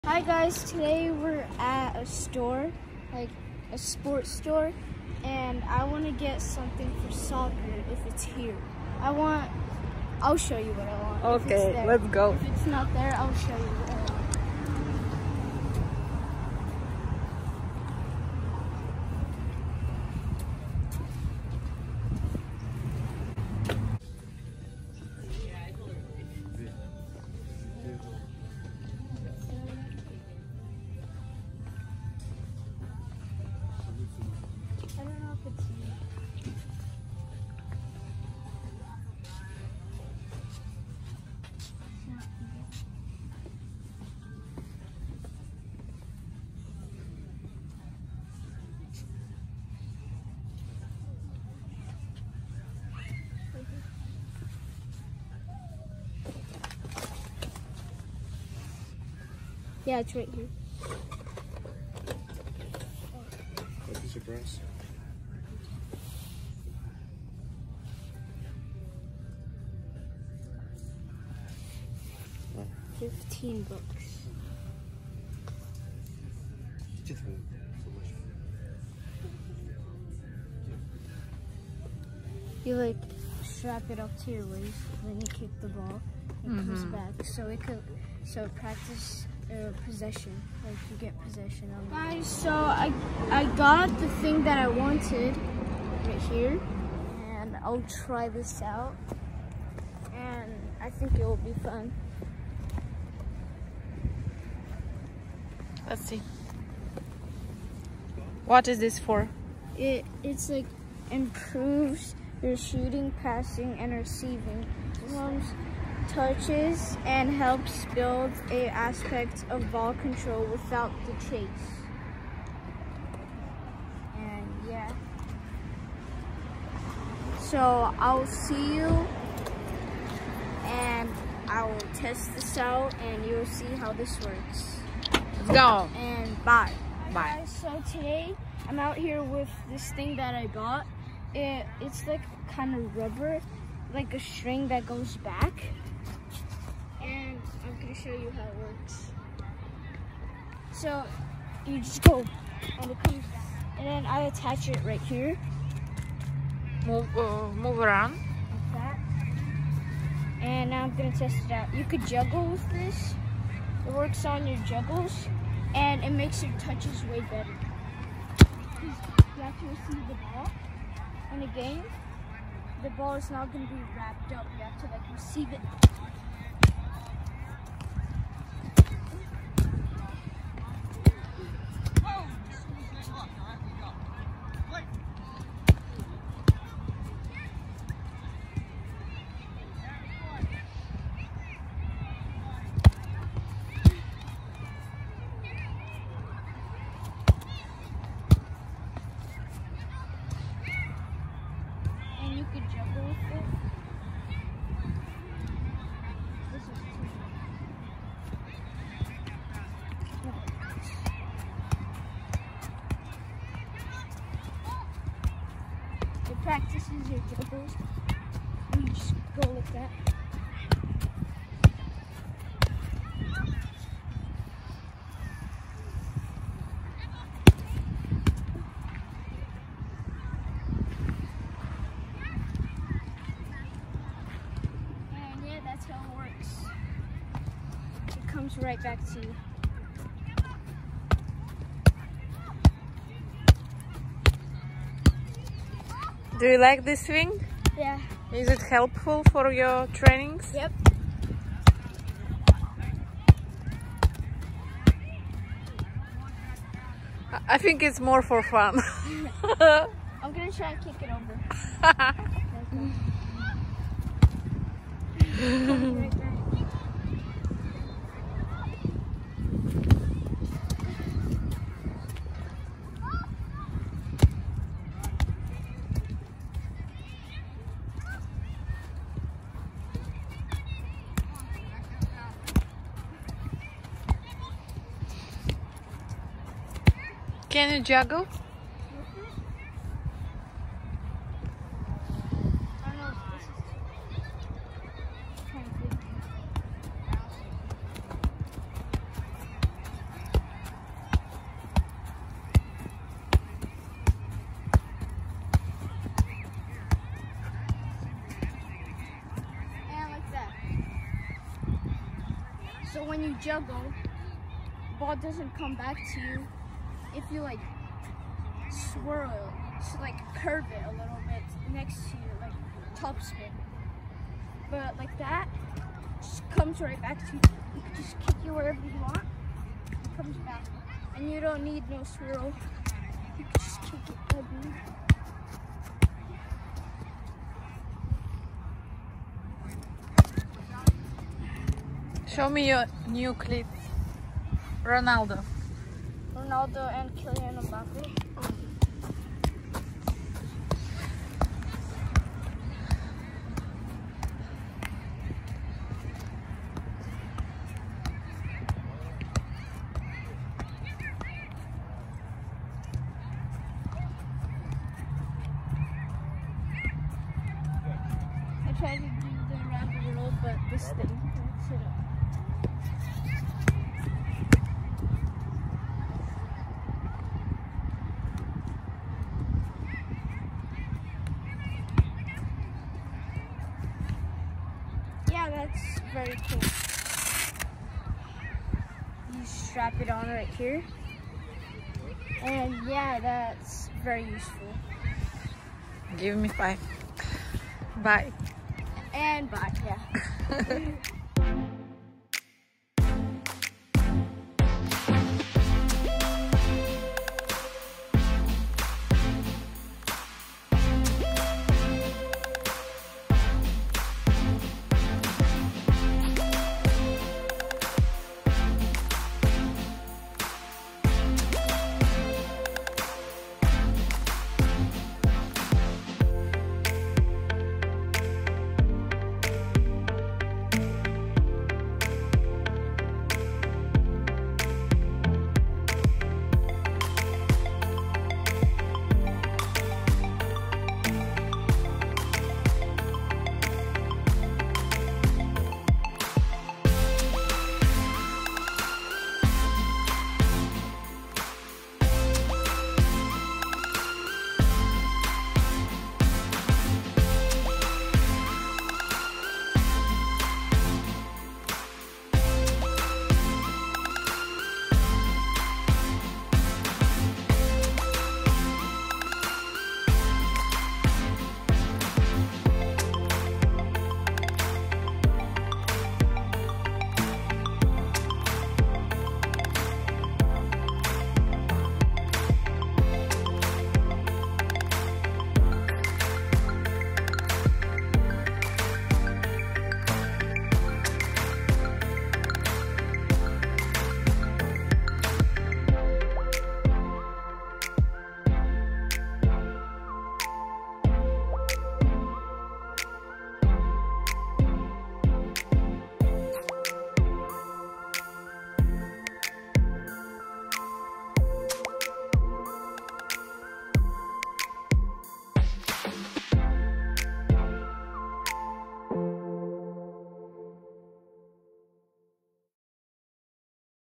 How I Practice With Star Kick Solo Soccer Trainer
Watch how I am practicing with Star Kick solo soccer trainer.